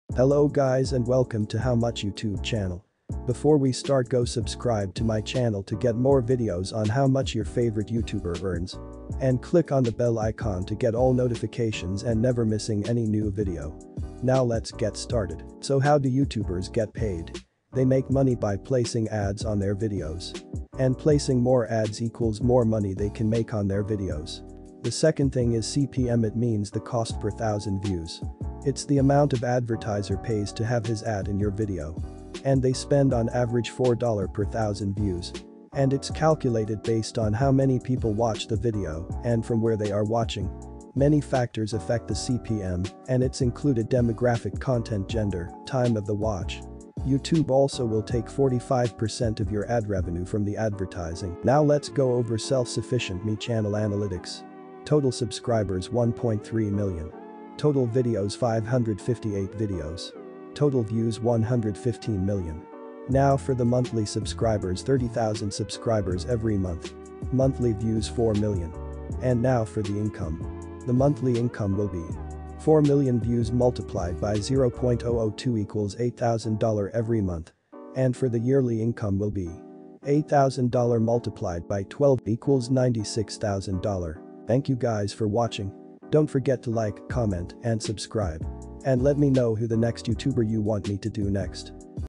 How Much Does Self Sufficient Me Make on YouTube
In this video, I will show you the estimate of how much money does Self Sufficient Me makes on YouTube.